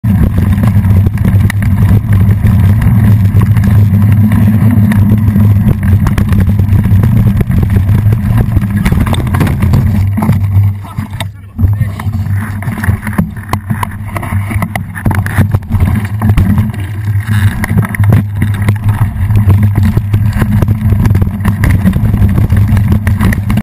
Crash on Stage 8 of ZGE
Zombie Goat Enduro, this little spill cost me precious seconds that bumped me from the podium =(

Hanlebar was crooked but finished out the stage.  Left turns were still fine, but right turns were wonky, heh